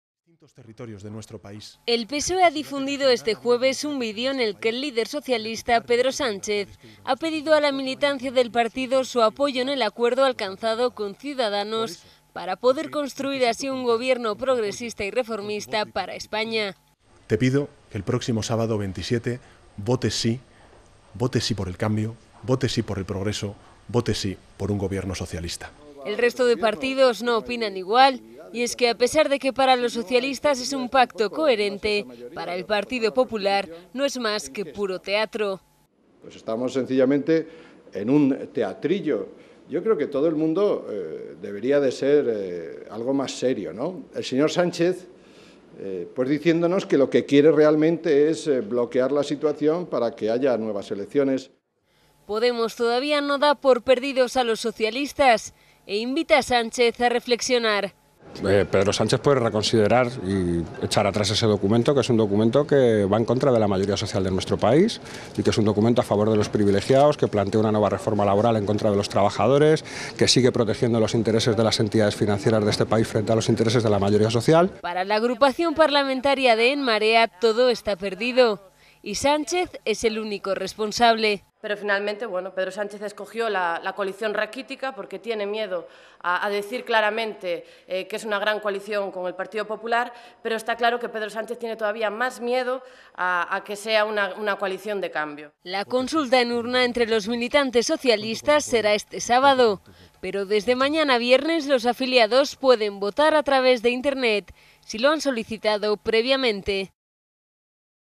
PP y Podemos se oponen al pacto de PSOE y C's
El PSOE ha difundido este jueves un vídeo, en el que el líder socialista, Pedro Sánchez, ha pedido a la militancia del partido su apoyo al acuerdo alcanzado con Ciudadanos para poder construir así un gobierno progresista y reformista para España.